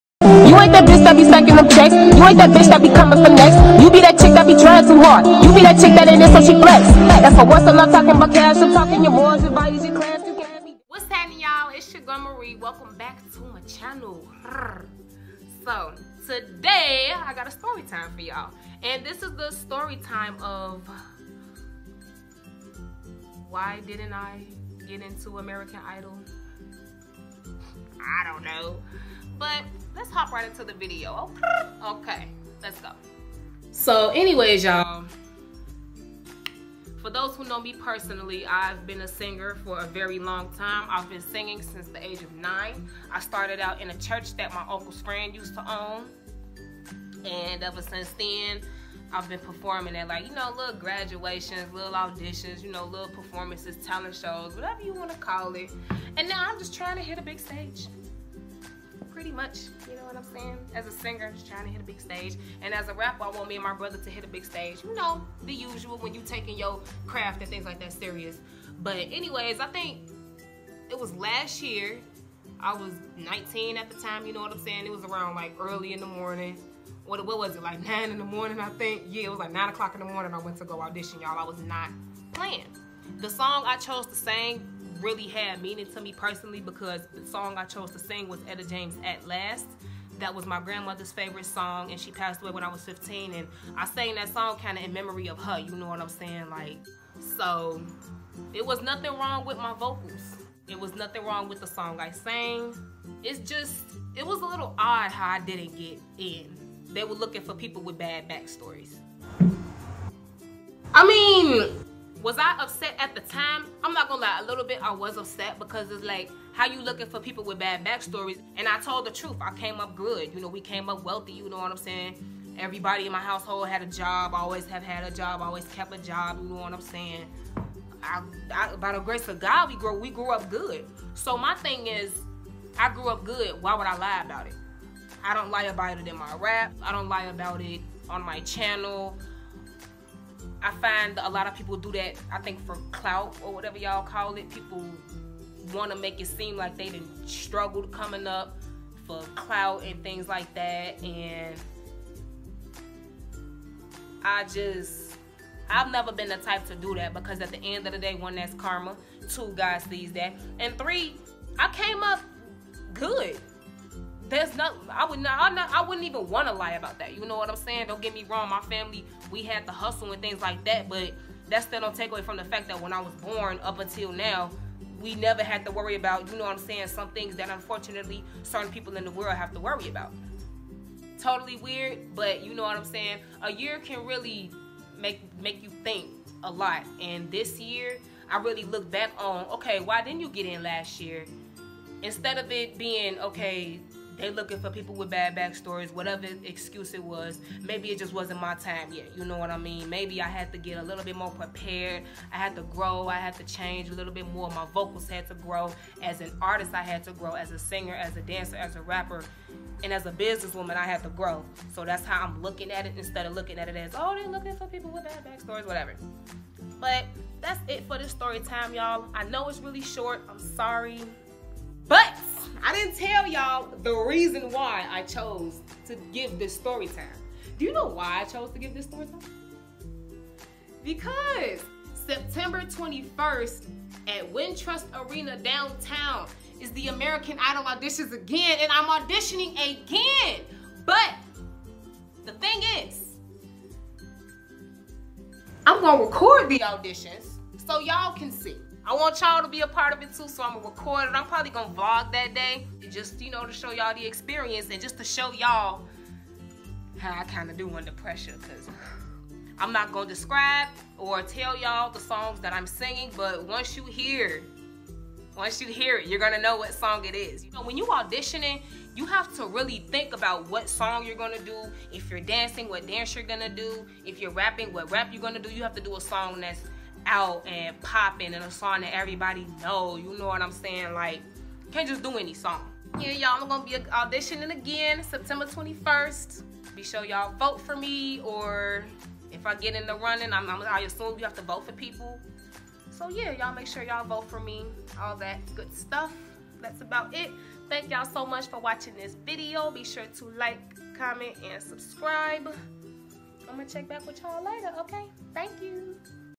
Story time: Why I didn’t get picked for American Idol
I shared this story time because It was an experience I really grew from. I want to show you guys that if at first you don’t succeed, TRY TRY AGAIN. If you still don’t succeed, Change the steps, never the goal.